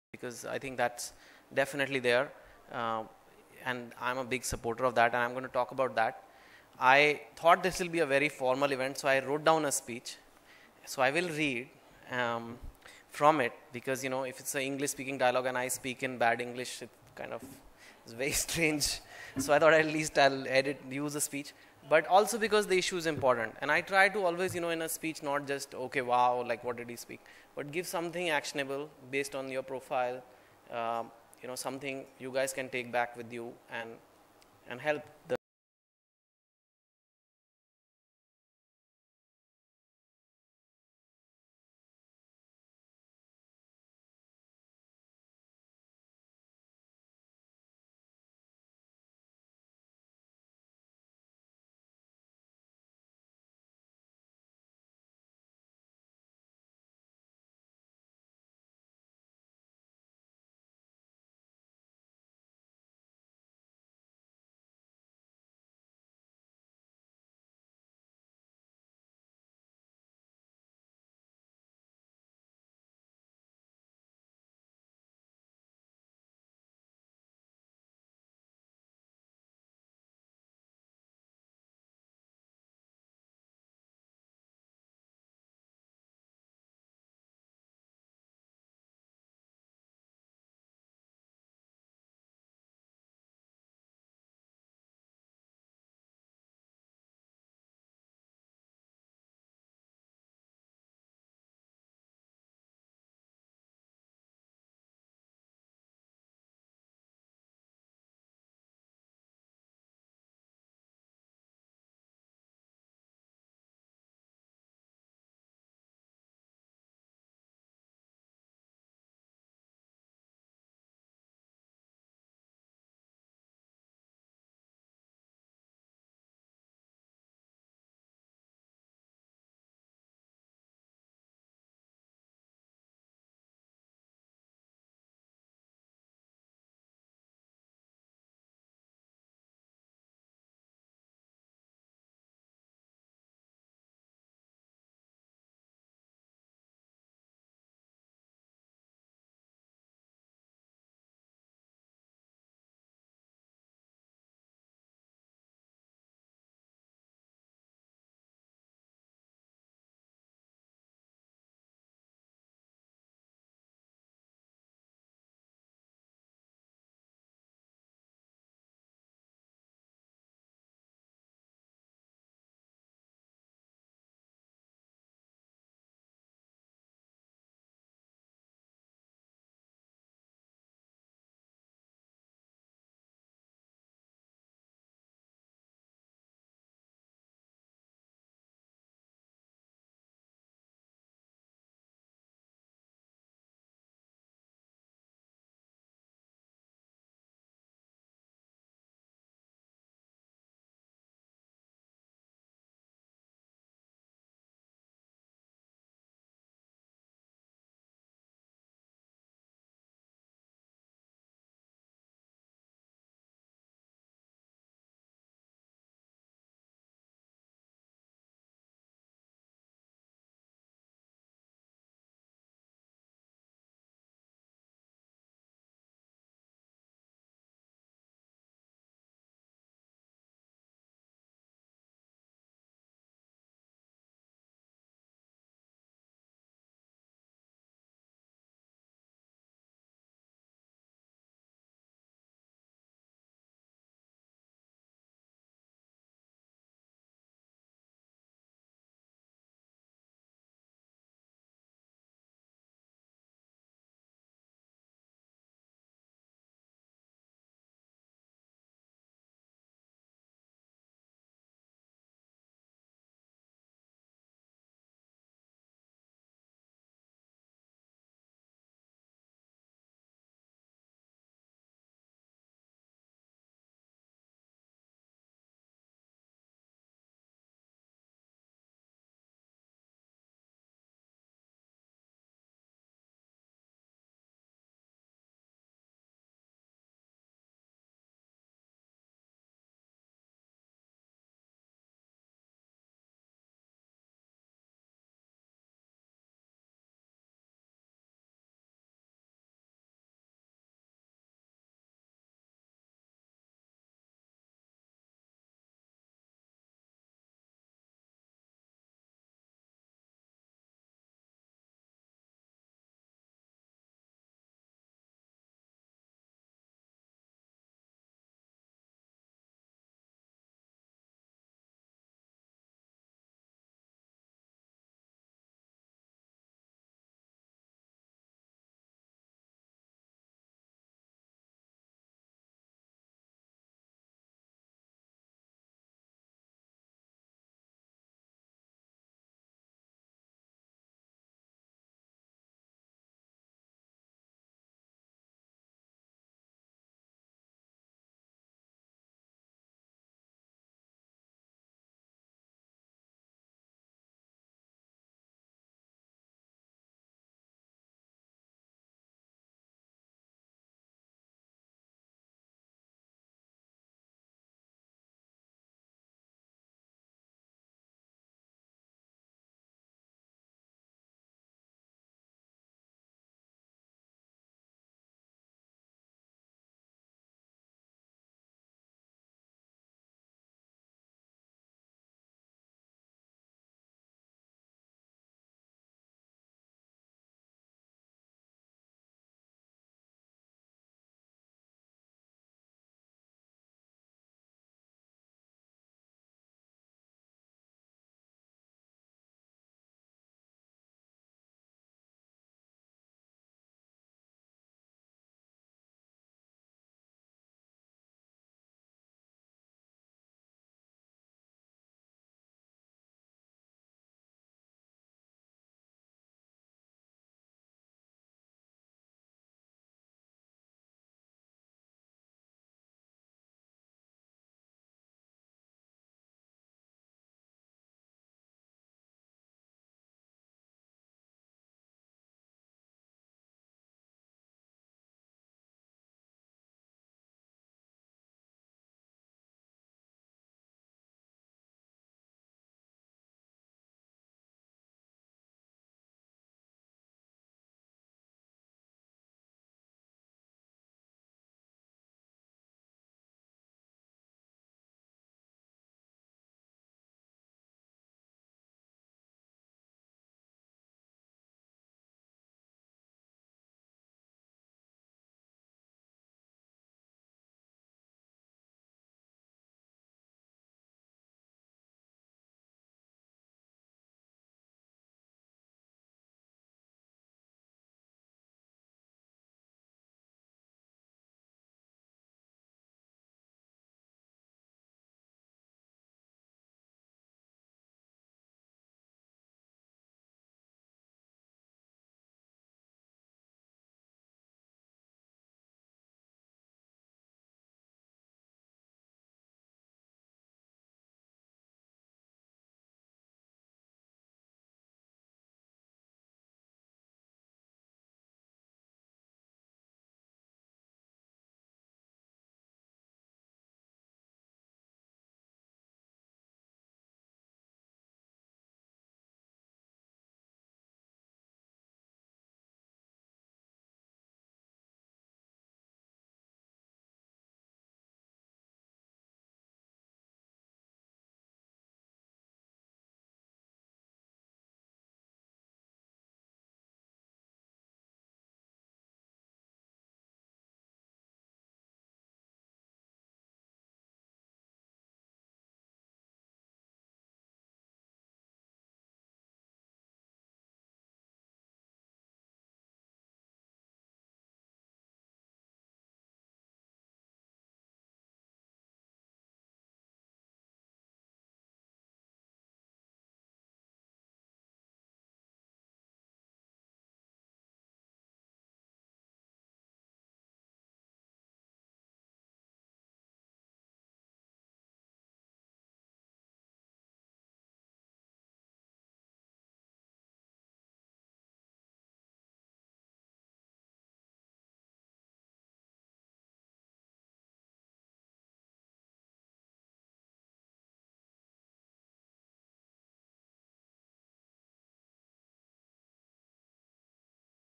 Chetan Bhagat speaks on what English language means to him (Part II)
Chetan Bhagat on English language and role of British Council